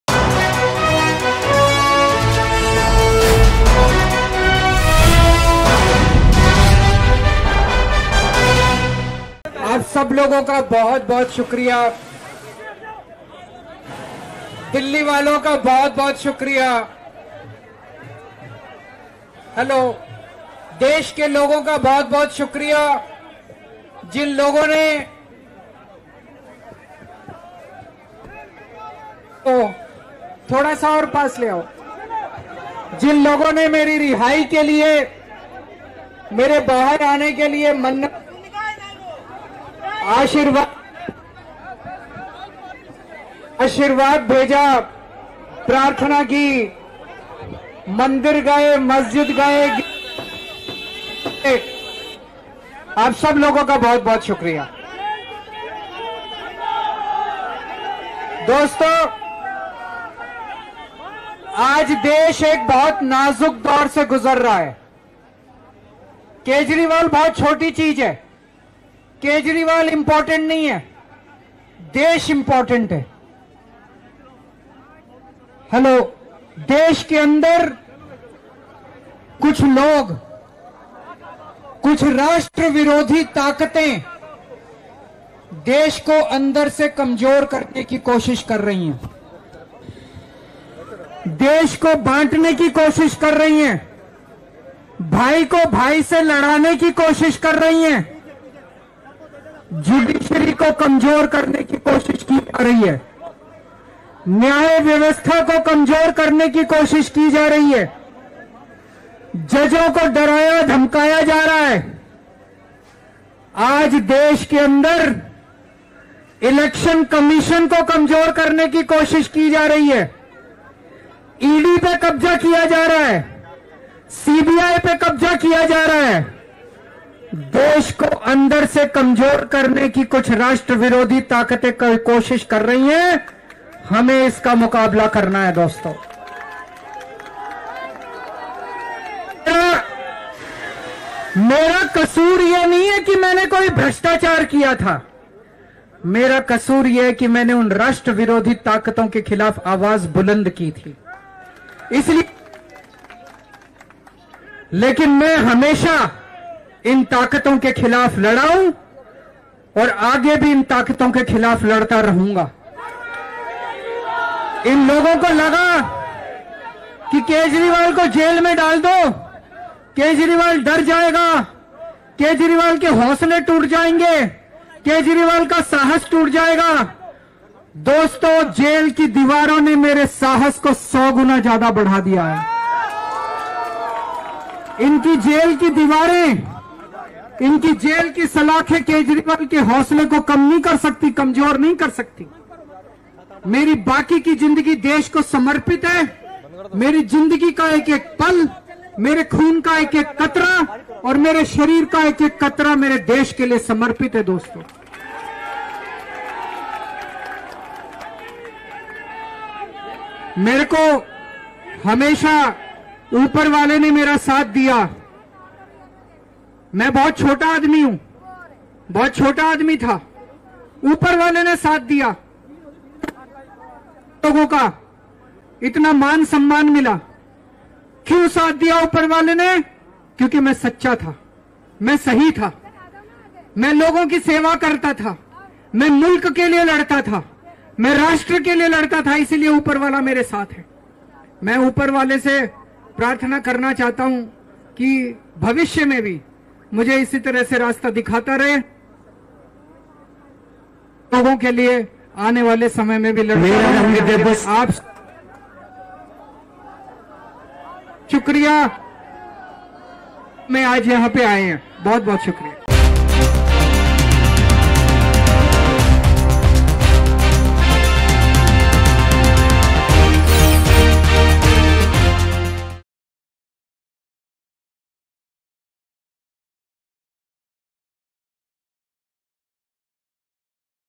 Arvind Kejriwal’s First Speech After Getting Bail | Aam Aadmi Party | Delhi | Political News
Arvind Kejriwal’s First Speech After Getting Bail. He has been released from Tihar Jail after the Supreme Court granted him bail in the Delhi excise policy case.